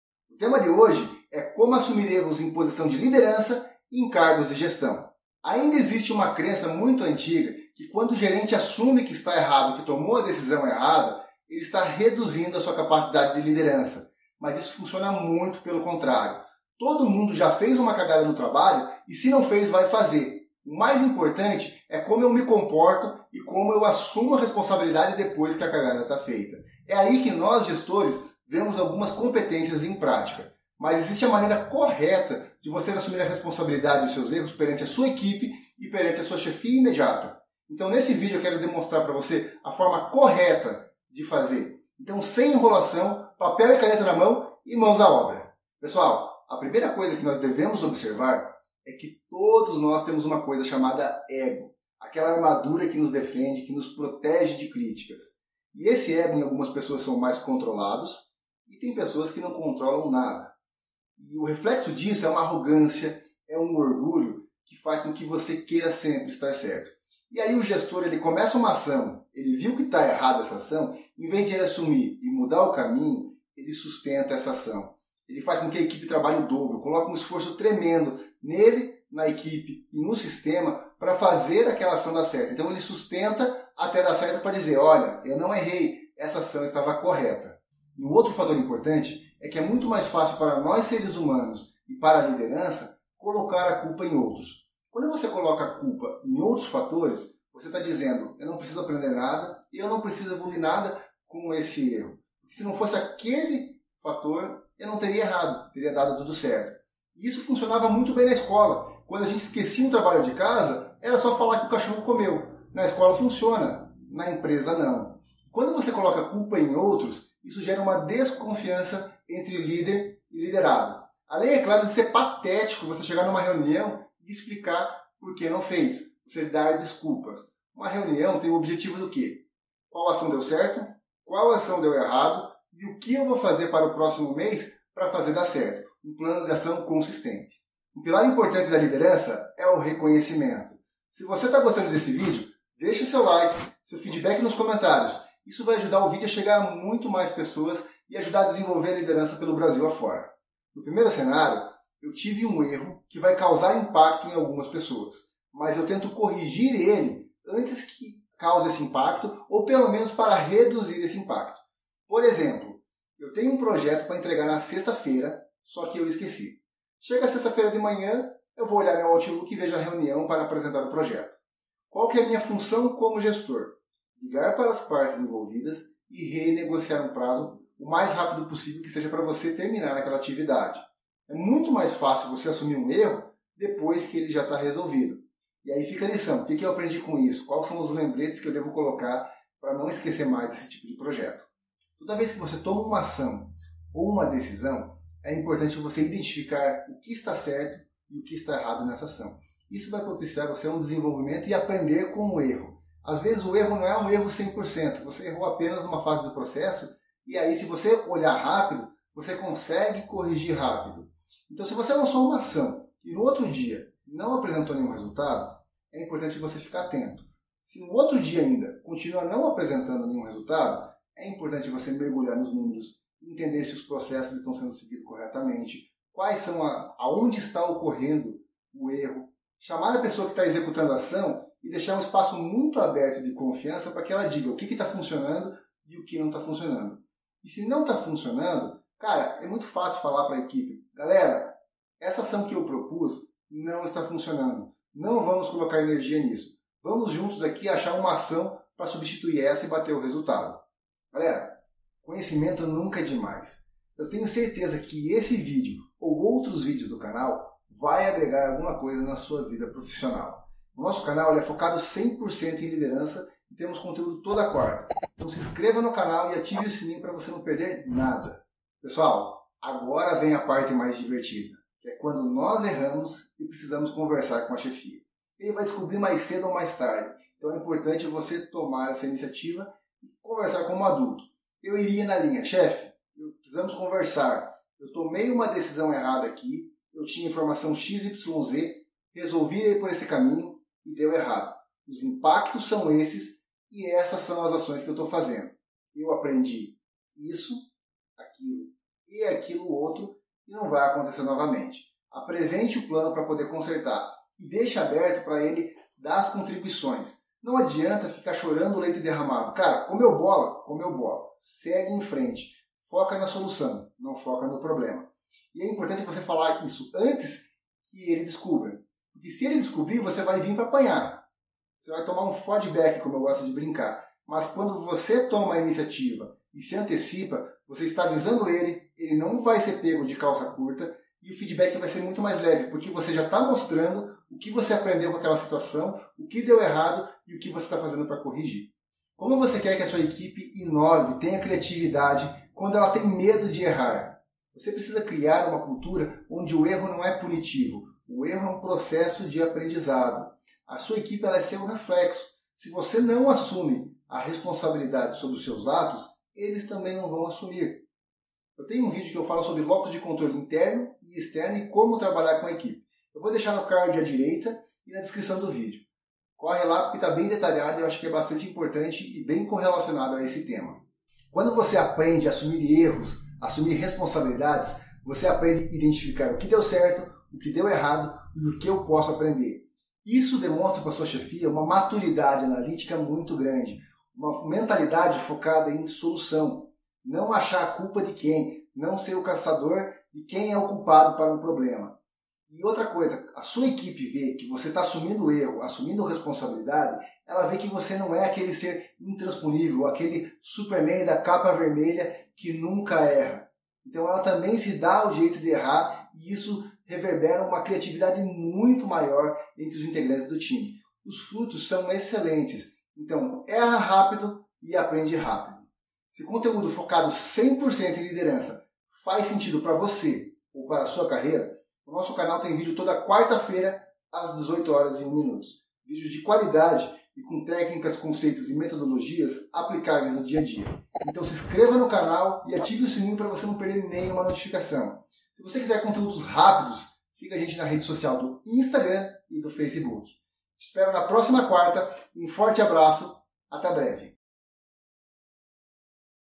O Líder Também Erra | Livre-se da Dificuldade em Assumir Erros!
Pessoal, precisamos ter claro que o líder também erra, assumir erros é uma virtude da gestão e a dificuldade de assumir erros é natural do ser humano, é muito mais fácil colocar a culpa em fatores externos do que assumir erros para o time, o nome desse conceito é lócus de controle.
O lócus de controle na liderança é uma das soft skills da liderança e obedecer as leis da auto responsabilidade, não assumir erros é um péssimo exemplo de gestão de pessoas, o mais importante que o erro é o comportamento após a falha, isso demonstra para nosso time que assumir erros no ambiente de trabalho é normal, não é punitivo, é um caminho para o acerto.
O lócus de controle para liderança possibilita você desarmar seu EGO e ser flexível em mudar de opinião quando não está correto, nesse vídeo eu te ajudo como assumir erros na gestão de pessoas sem perder credibilidade na liderança.
O Lócus de controle na gestão traz confiança entre liderança e chefia, pois o chefe não é pego desprevenido quando você se antecipa para levantar um problema trazendo auto responsabilidade na liderança e lócus de controle na gestão de pessoas.

Se conteúdo sobre liderança faz sentido para seu desenvolvimento, você esta no lugar certo!

Forte Abraço!!! 👊👊👊